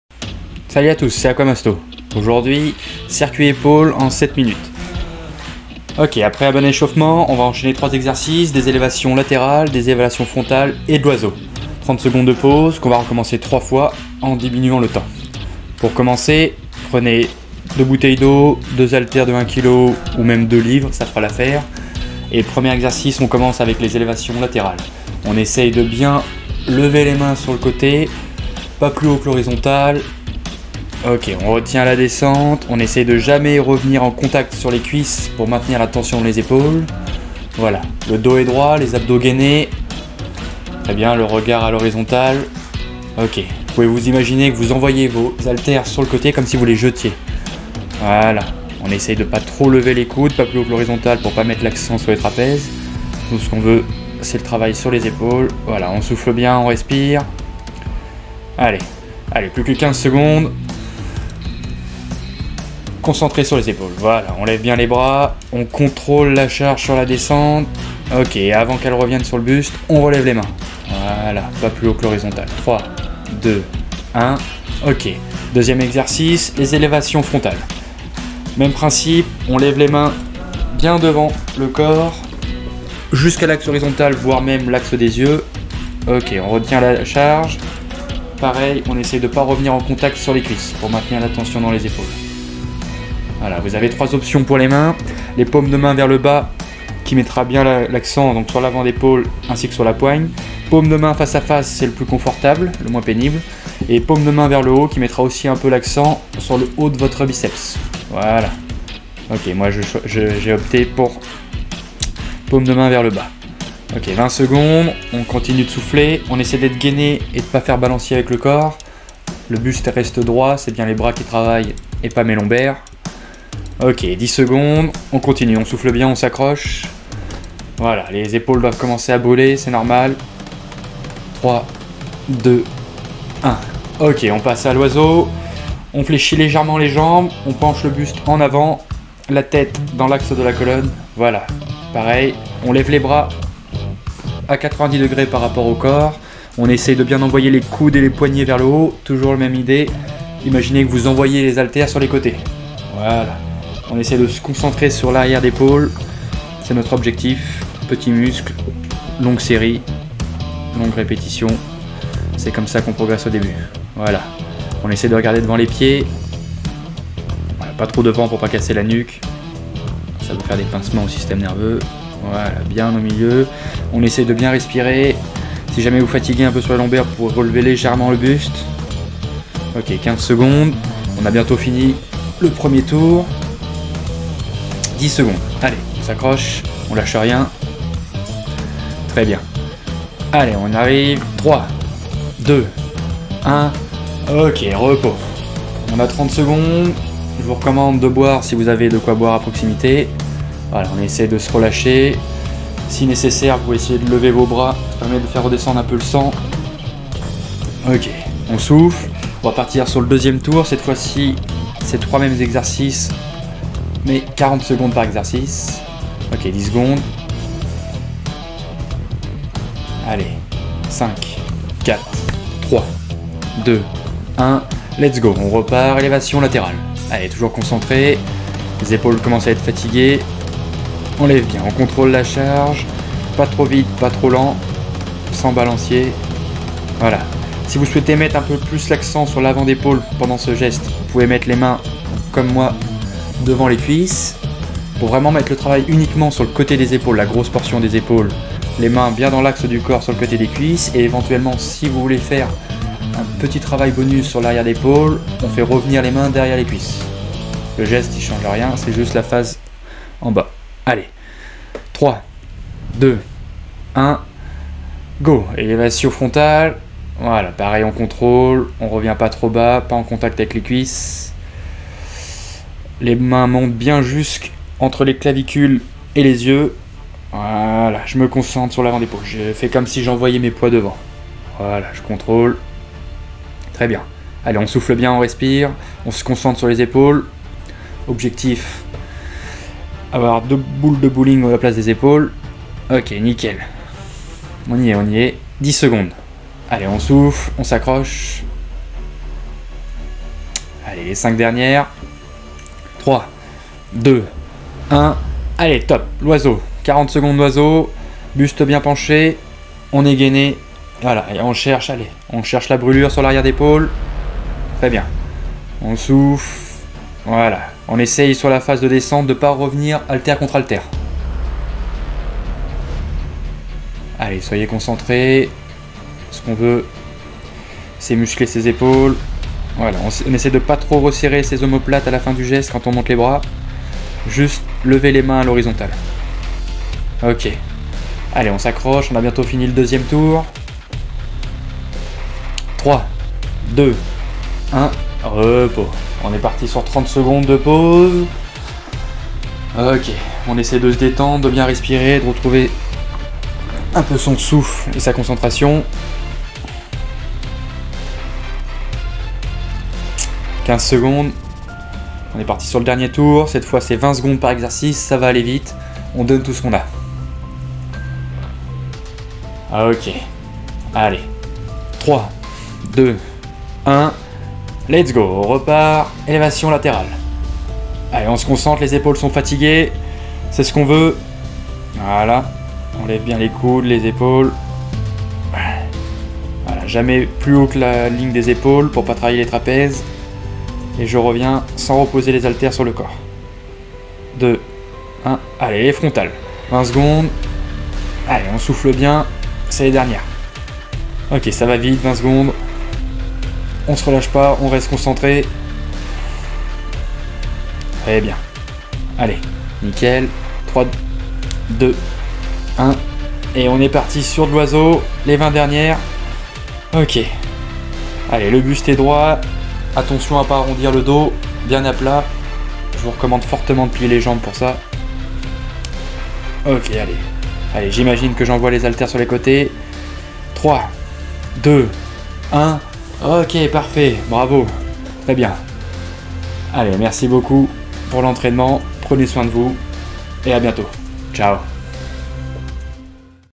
Entraînement des épaules à la maison EN 7 MINUTES !!! (peu de matériels)
Salut à tous, c'est Aquamasto

Séance épaules à la maison avec peu de matériel (1kg par bras seulement) en 7 minutes top Chrono !

Ensuite on enchaîner 3 exercices de bases pour les épaules à savoir des élévations latérales, des élévations frontales et de l'oiseau avec le buste penché.

Déroulement de la séance :

1minute d'élévations latérales sans pause
suivis d'1 minute d'élévations frontales sans pause
suivis d'1minutes d'oiseau sans pause
30 secondes de repos
et on repars sur 40secondes d'élévations latérales
puis 40secondes d'élévations frontales
puis 40secondes d'oiseau
30 secondes de repos
et enfin 20s d'élévations latérales
20s d'élévations frontales
20s d'oiseau

Voila  de quoi bien faire chauffer vos épaules pour de bonnes sensations !!

bonne séances à vous

Ciao